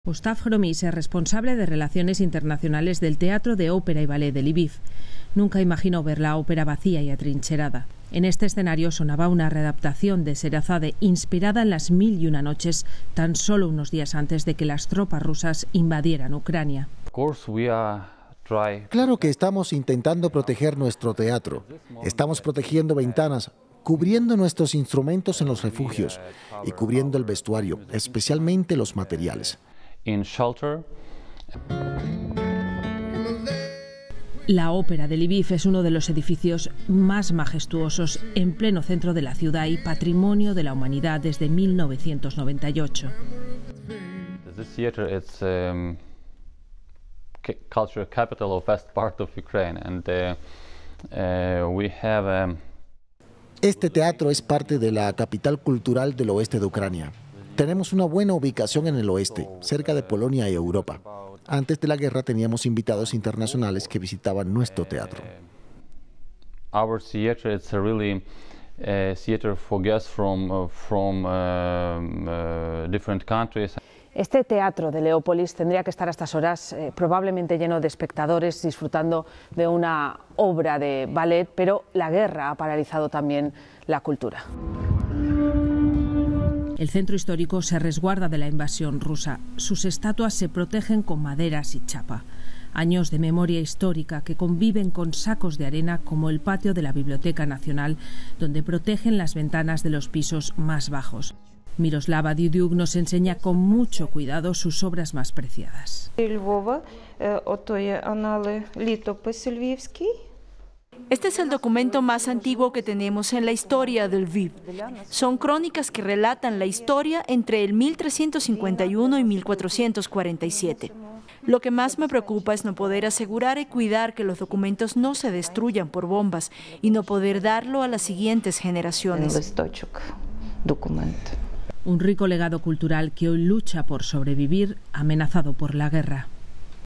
Ucrania busca proteger su patrimonio cultural del ataque ruso • FRANCE 24 Español
Martirizada por el asedio ruso, Ucrania también teme por la integridad de su rico patrimonio cultural. En Lviv, las autoridades intentan proteger iglesias, teatros, bibliotecas y estatuas que forman parte del patrimonio mundial de la Unesco. Informe especial de Leticia Álvarez e Irene Savio.